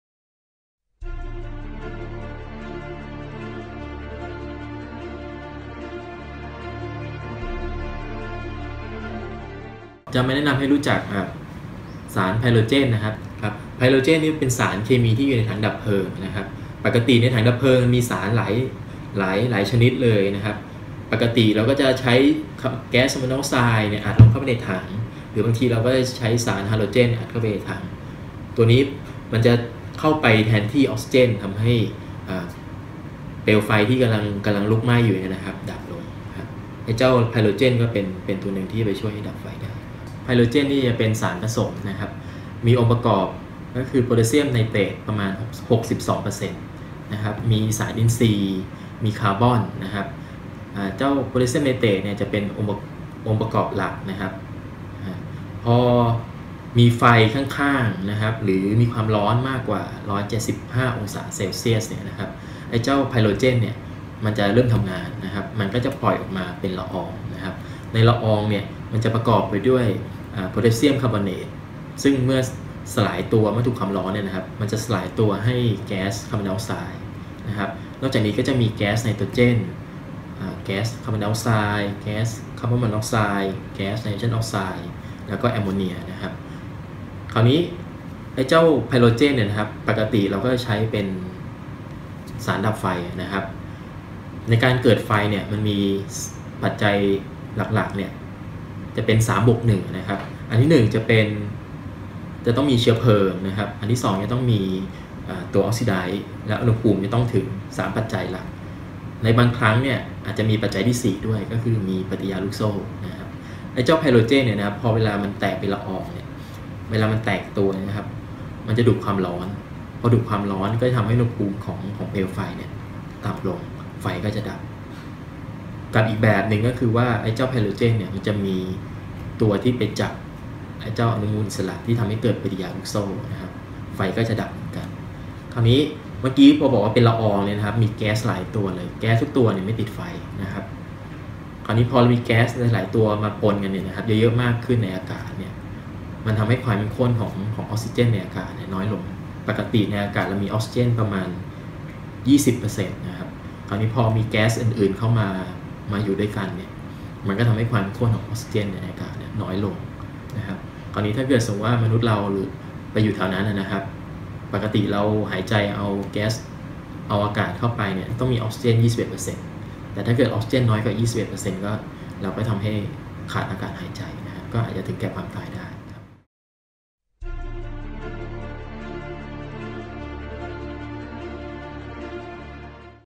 ทำความรู้จักกับสารดับเพลิง Pyrogen
โดย รองศาสตราจารย์ ดร. เอกสิทธิ์ สมสุข

คณะวิทยาศาสตร์ มหาวิทยาลัยมหิดล